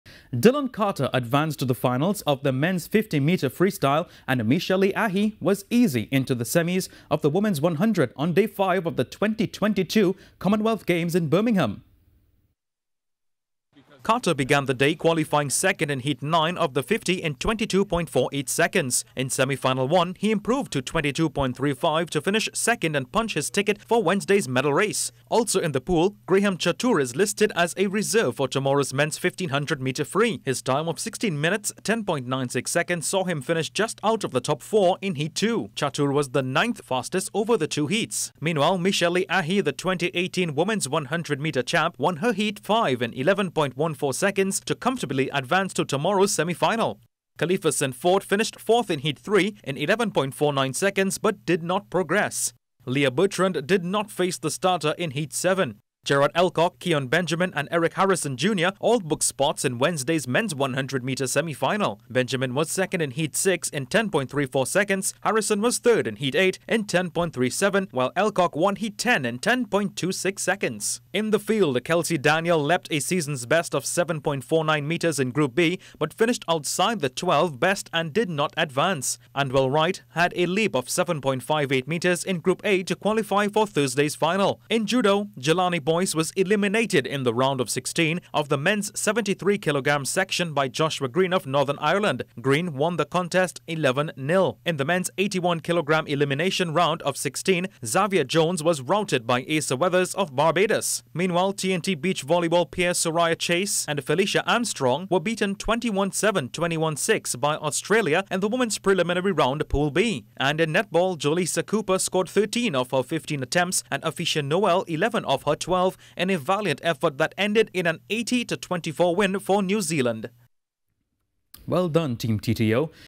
Commonwealth Games Day 5 - T&T Advances In Men's 50M Butterfly, Women's 100M
Dylan Carter advanced to the Finals of the Men's 50 Metre Freestyle, and Michelle Lee Ahye was easy into the Semis of the Women's 100 on Day 5 of the 2022 Commonwealth Games in Birmingham.